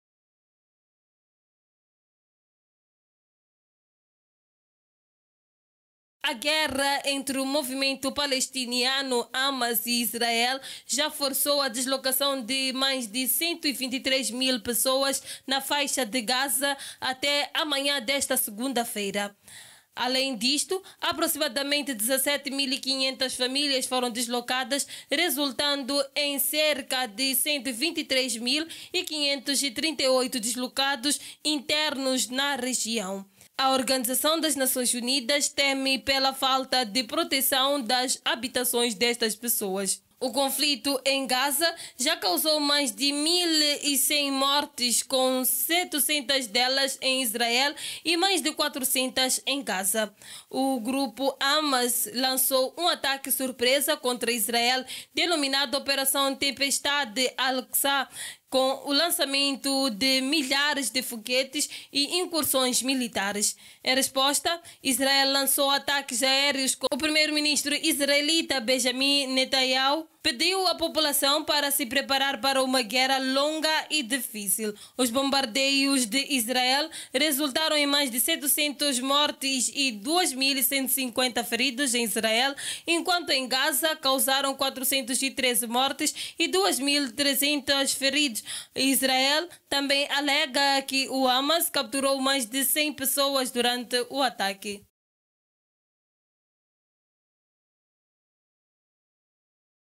CONFLITO ISRAELO PALESTINIANO ONU DIZ QUE HÁ 123 MIL DESLOCADOS NA FAIXA DE GAZA
O número de deslocados na Faixa de Gaza aumentou para 123 mil devido ao conflito entre Israel e o Hamas. Mais de 1.100 pessoas morreram em dois dias de confrontos, incluindo 700 em Israel e mais de 400 em Gaza.